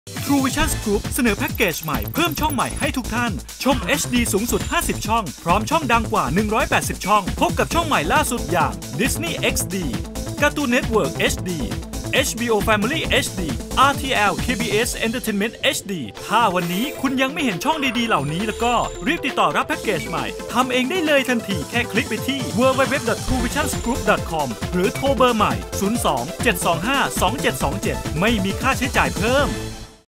ทรูวิชั่นส์ กรุ๊ป เสนอแพ็กเกจใหม่เพิ่มช่องใหม่ให้อีกเพียบ
ชมเอชดีสูงสุด 50 ช่อง พร้อมช่องดังกว่า 180 ช่อง
พบกับช่องเอชดีใหม่ล่าสุด อย่าง
Disney XD, Cartoon Network HD, HBO Family HD, RTL CBS Entertainment HD, Fashion One, Fashion TV, MTV HD, BBC World News, True Sport HD3, Edge Sport, Trace Sport และอีกเพียบ

สมาชิกเดิมถ้ายังไม่เห็นช่องดีๆ แบบนี้ รีบขอเพิ่มช่อง ไม่ต้องจ่ายเพิ่ม แค่คลิ๊กไปที่ หรือโทรเบอร์ใหม่ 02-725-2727
สำหรับผู้ที่สนใจแพ็กเกจต่างๆ ของทรูวิชั่นส์ ติดต่อ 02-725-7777 หรือที่ ทรูช้อป ทรูพาร์ทเนอร์ทั่วประเทศ